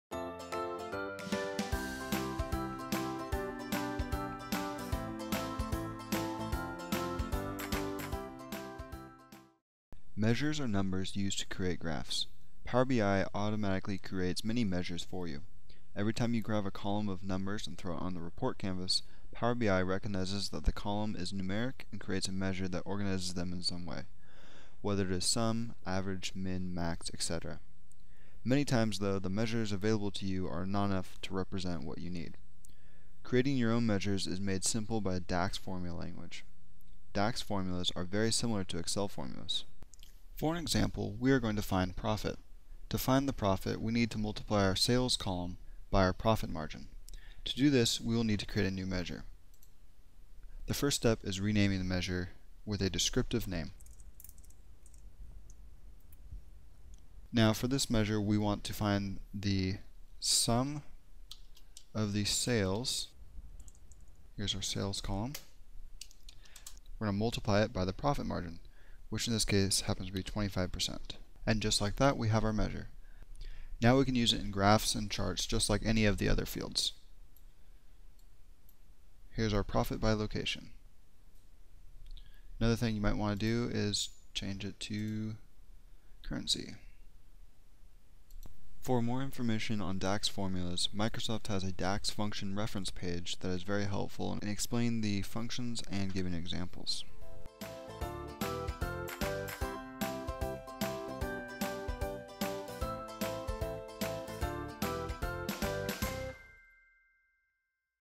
Measures
How to create calculated measures in Power BI Desktop.